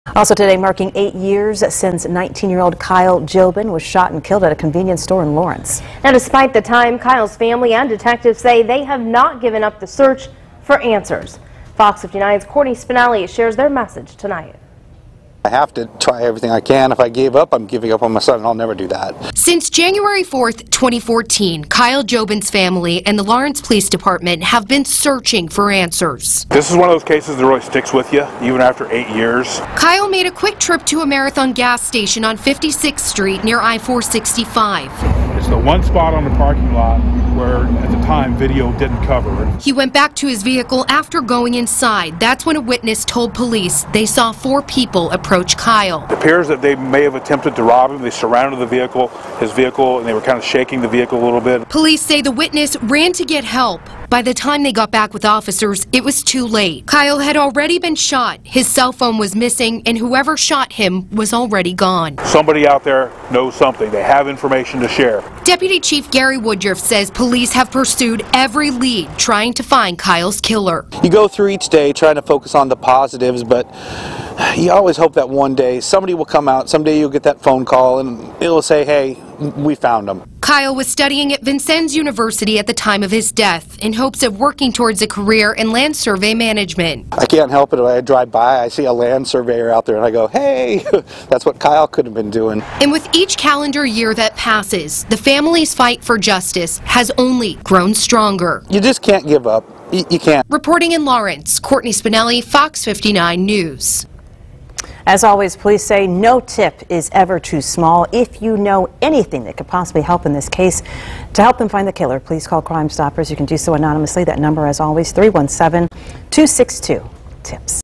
Family, police push for answers 8 years after murder of Vincennes University student in Lawrence
Tuesday marks eight years since a shooting at a Lawrence convenience store that ultimately claimed the life of 19-year-old Kyle Jobin.